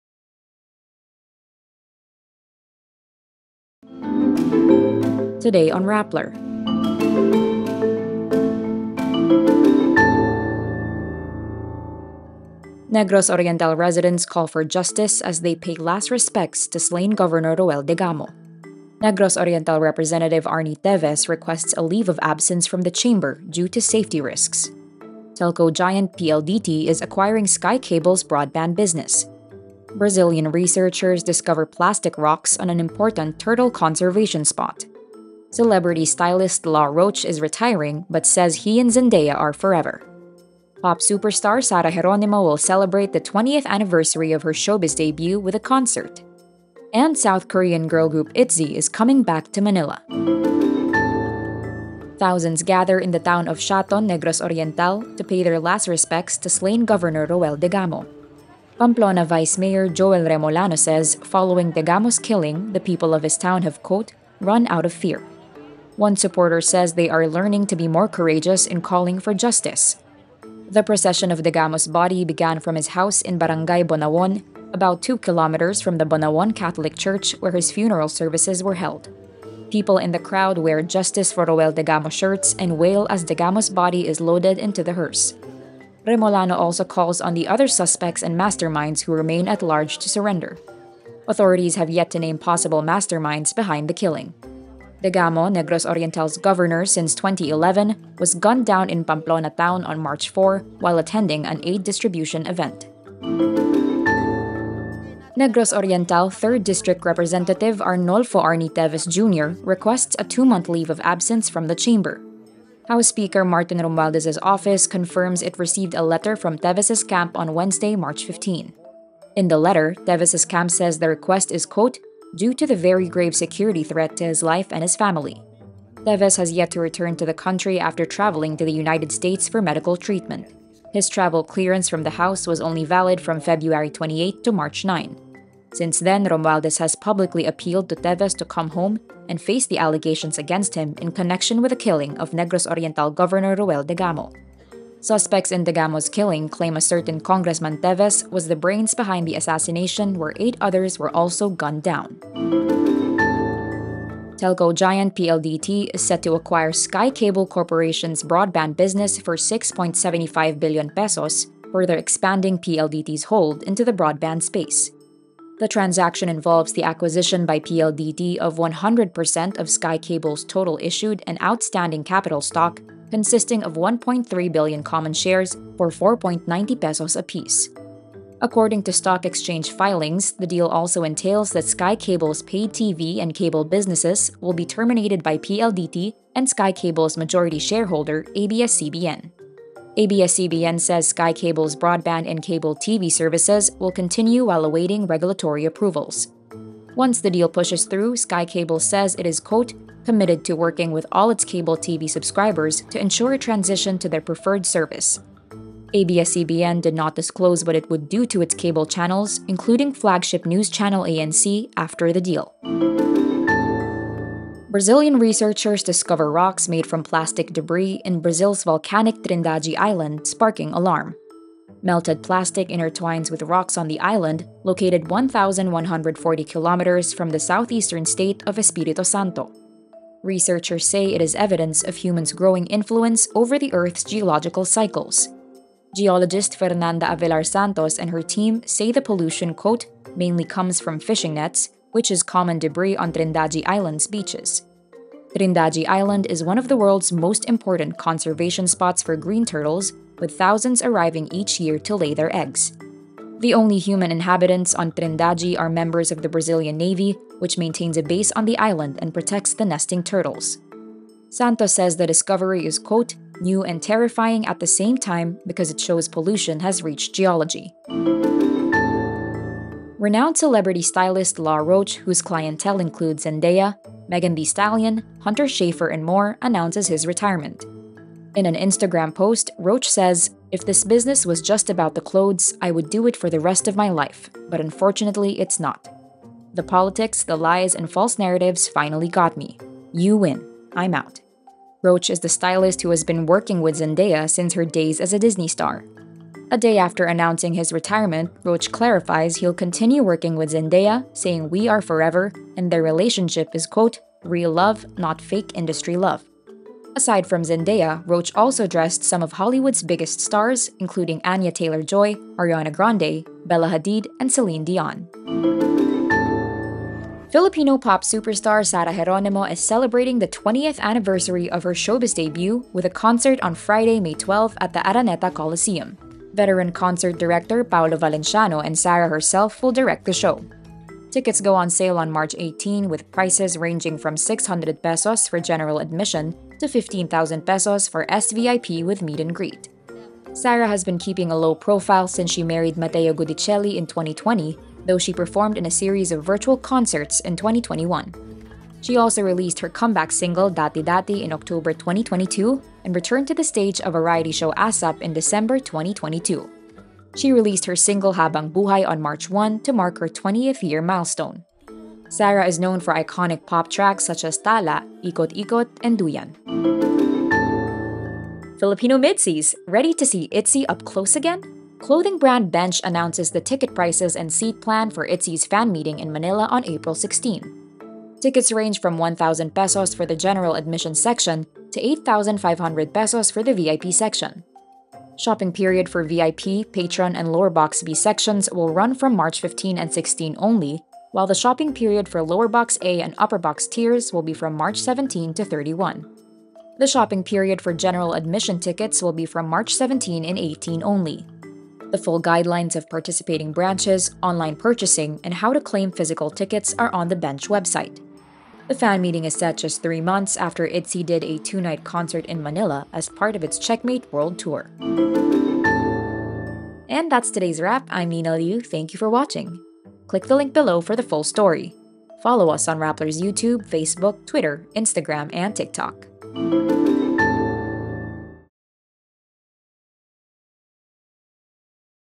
Negros Oriental governor Degamo laid to rest | The wRap
Today on Rappler – the latest news in the Philippines and around the world:
- Negros Oriental residents call for justice as they pay last respects to Degamo
- Embattled Teves seeks 2-month leave from House due to ‘safety risks’
- PLDT acquiring Sky Cable’s broadband business for P6.75 billion
- Brazilian researchers find ‘terrifying’ plastic rocks on remote island
- ‘You win…I’m out’: Celebrity stylist Law Roach announces retirement
- Sarah Geronimo celebrates her 20th anniversary with concert
- Ticket prices, seat plan: ITZY’s fan meeting in Manila